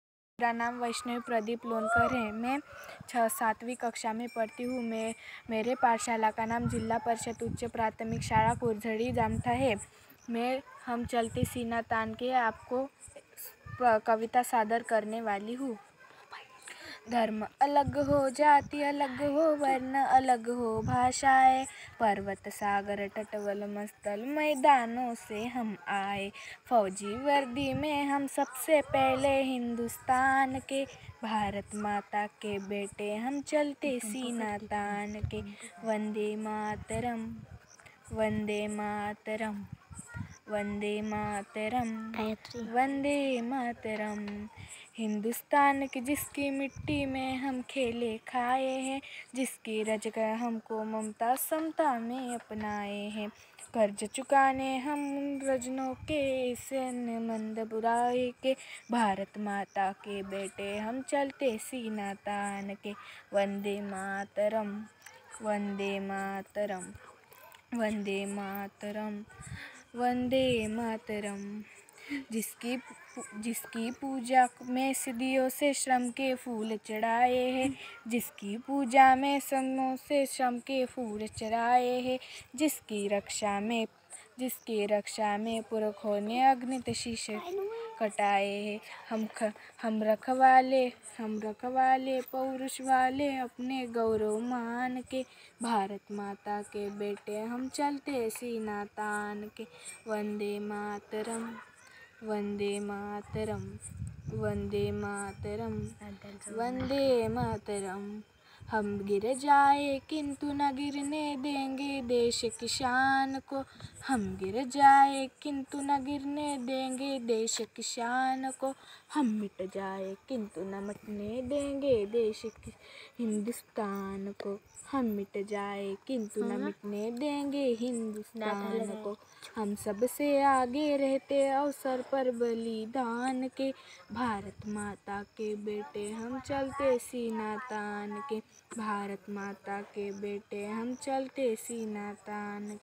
वैष्णवी ---- इ 7 -कविता सादरीकरण वीजि.प.ऊ.प्रा.शाळा,कुरझडी (जामठा)जि.वर्धा राज्य:- महाराष्ट्र
Video from Swati Ahire